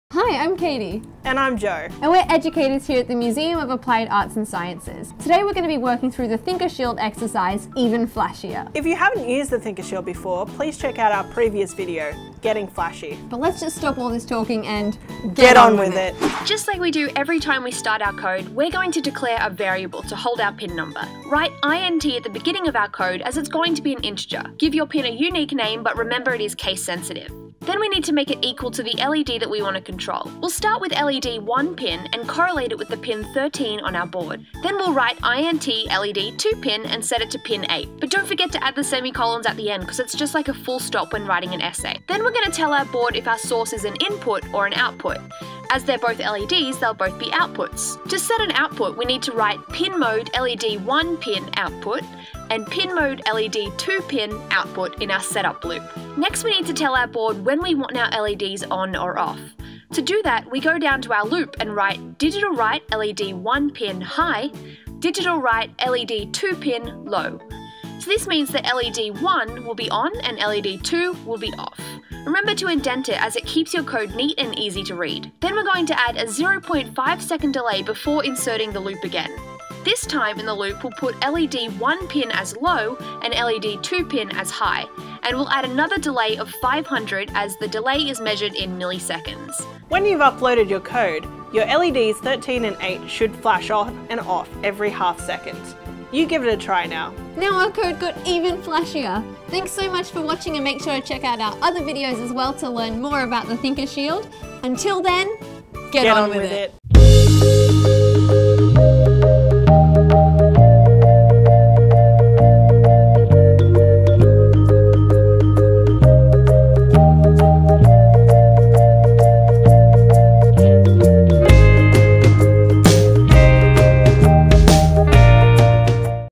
Even.Flashier (ThinkerShield)
Your ThinkerShield has six LEDs (on digital pins D8-D13). With some simple changes to your program code, you can easily control them. Let's start with two of them!